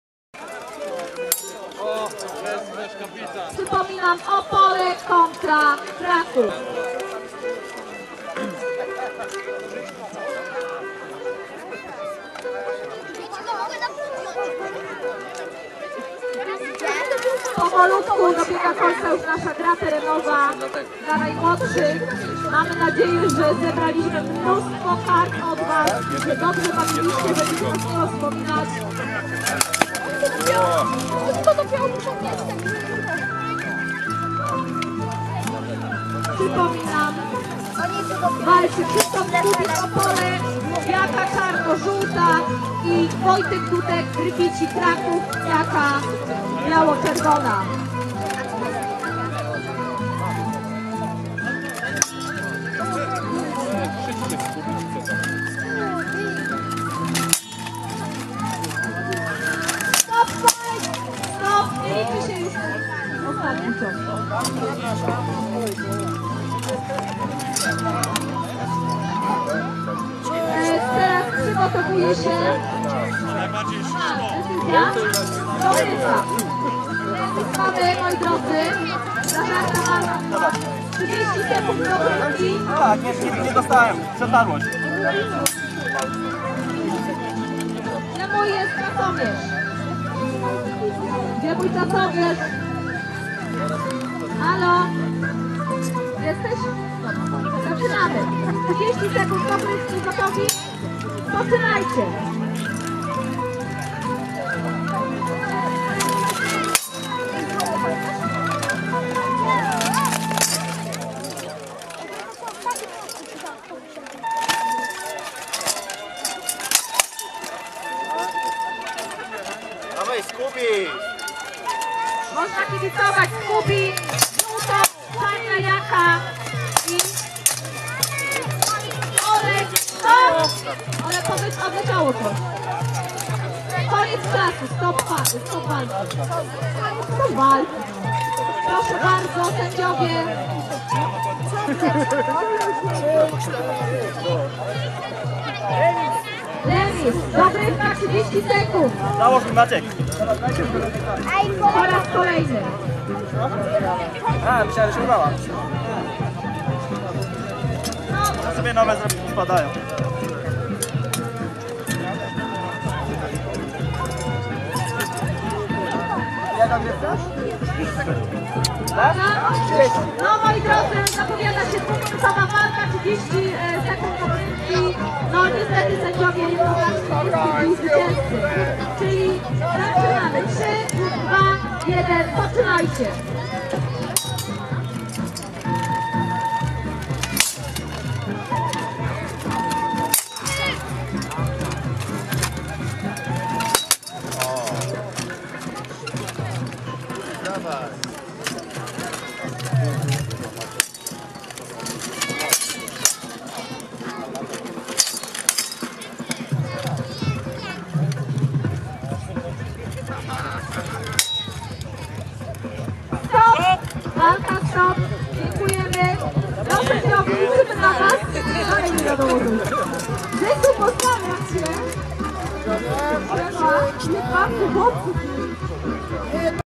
Aleksander Wędrychowski vs Krzysztof Skubis
Turniej miecza długiego.
Dolnośląski Festiwal Czasów Dawnych.
Plaża Gorzuchów 2016.